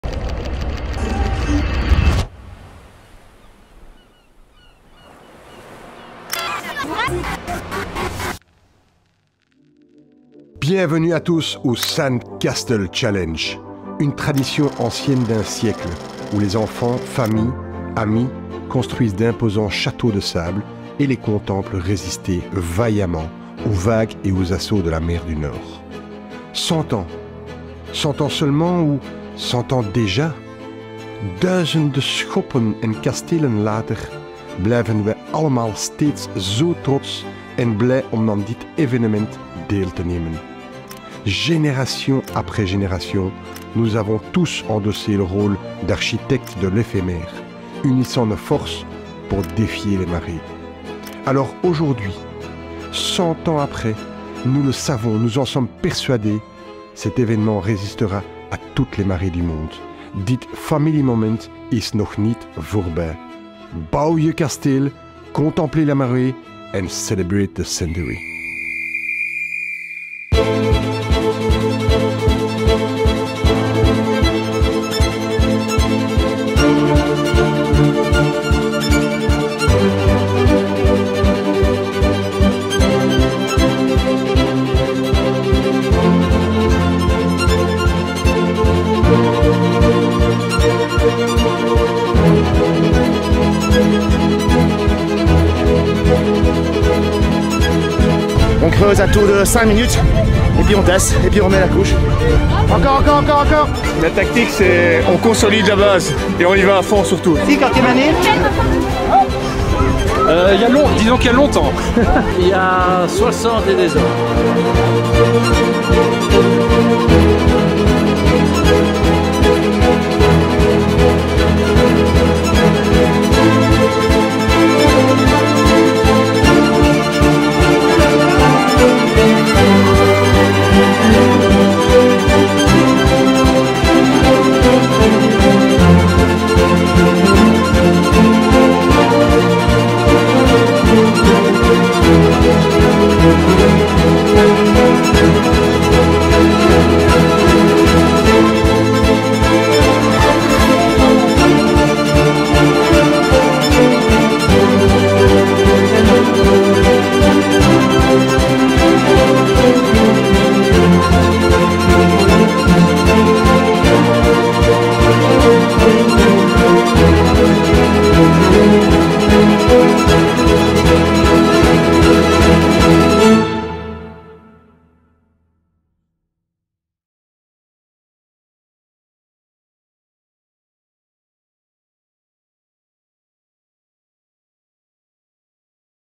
ZOUTE SANDCASTLE CHALLENGE 23 - Aftermovie
Relive the Belfius Zoute Sandcastle Challenge's centenary celebration in images as if you were there!
Historical event taking place in Knokke-Heist, which this year occurred on Sunday, August 13, 2023.